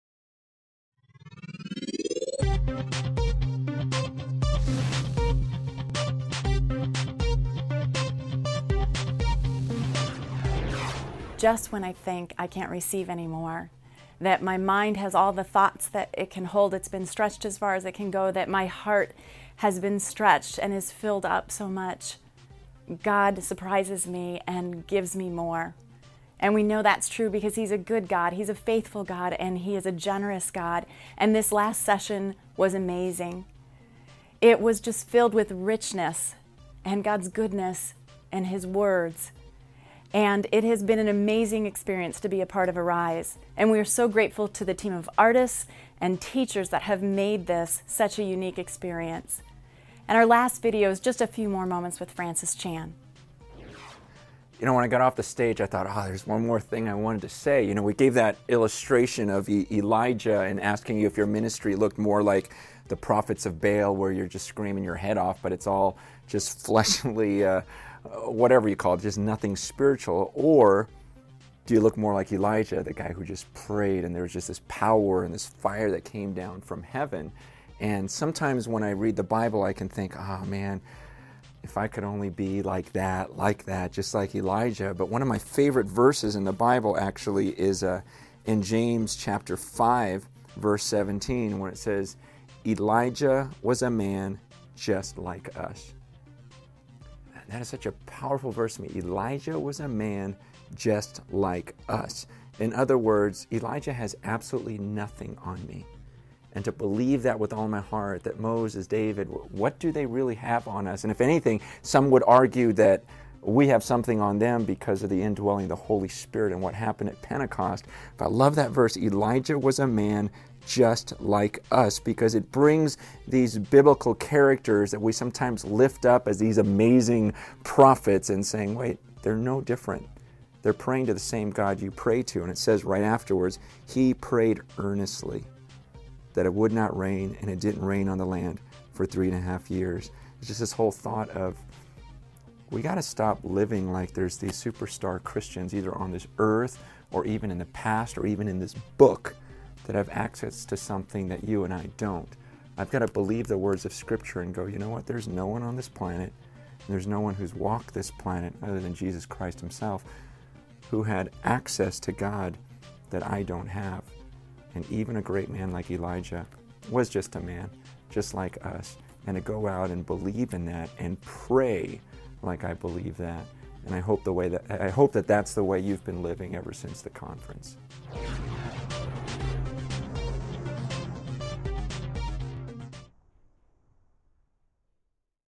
Willow Creek Arts Conference 2008 - Francis Chan
Francis Chan, Pastor of Cornerstone Church in Simi Valley, California, talks after session five of "Arise", the Willow Creek Arts Conference in June 2008.